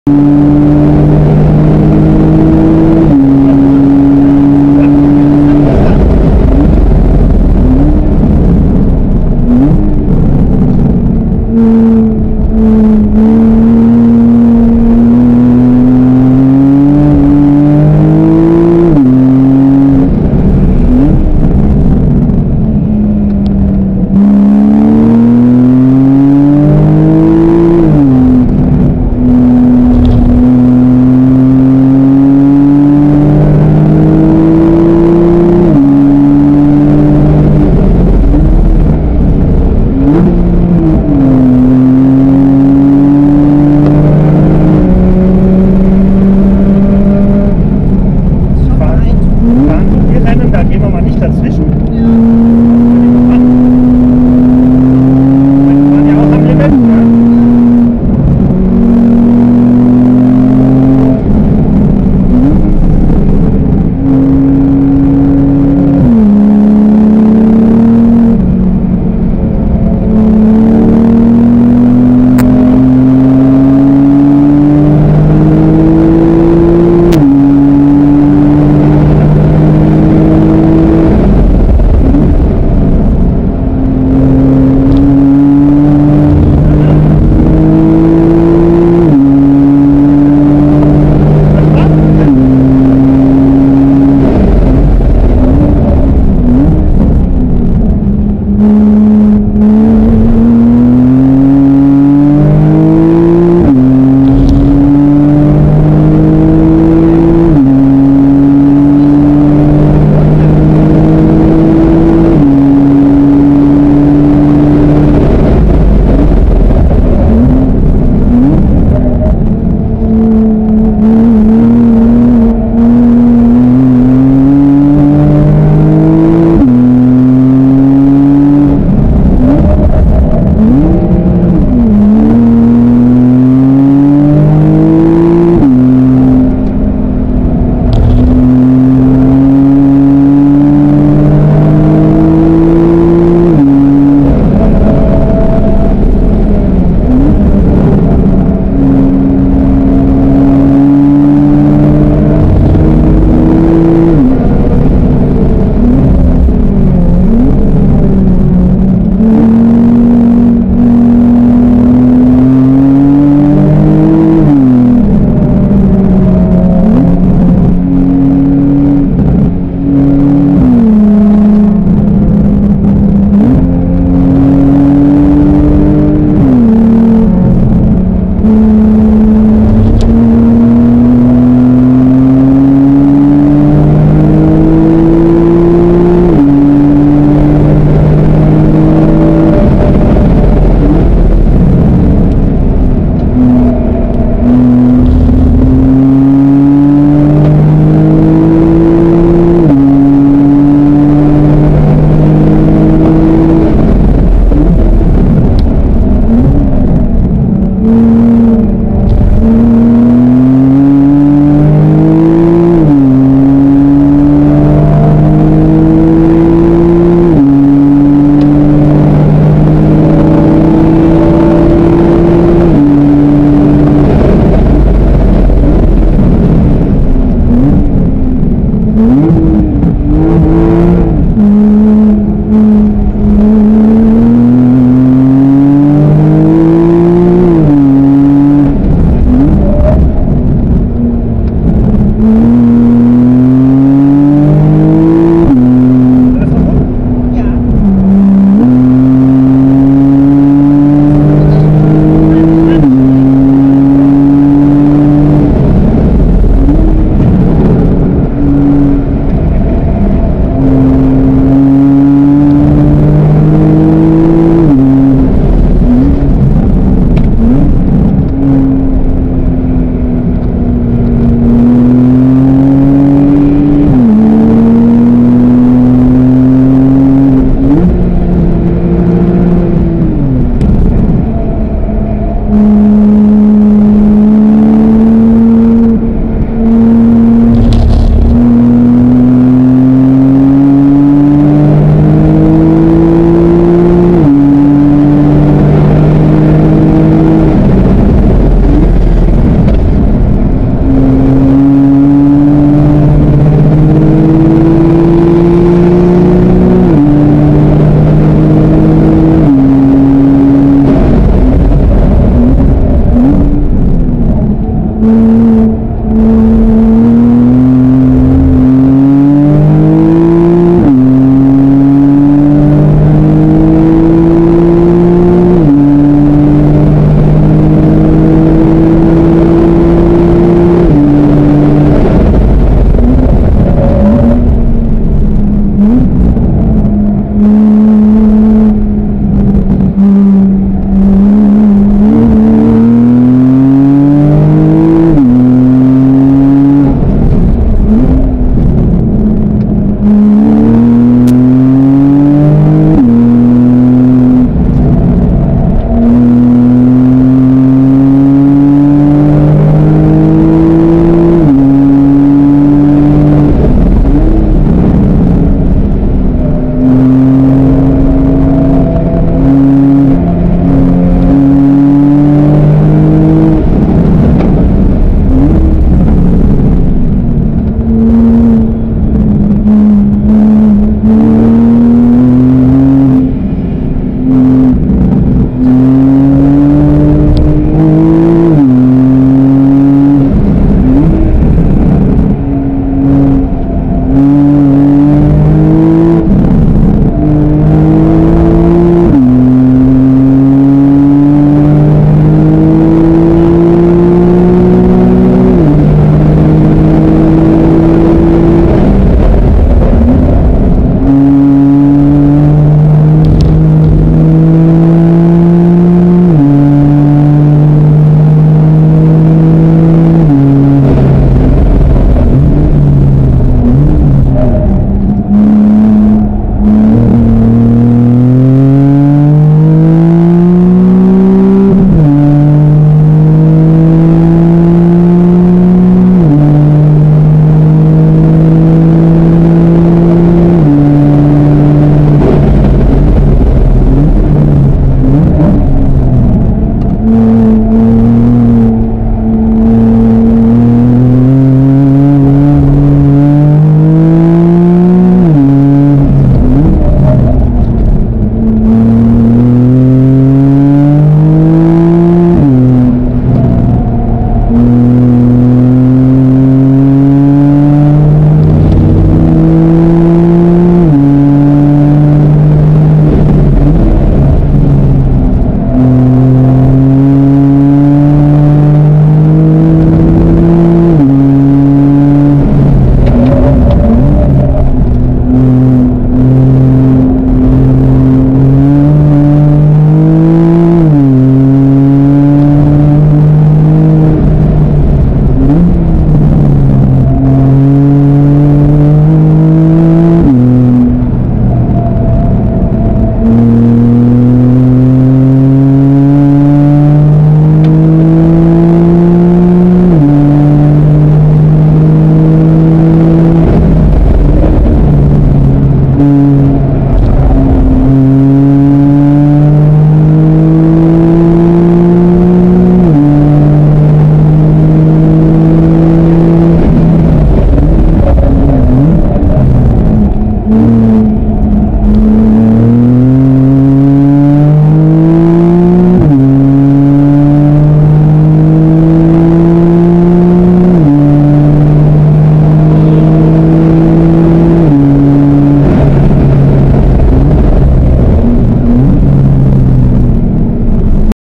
Porsche 991 C2S Hockenheimring Ausblasen 30 10 2016 PT3
Porsche 991 Carrera 2S PDCC mit Serienreifen
Bremsanlage GT3 RS , Fahrwerk KW V3, Setup Wolfgang Weber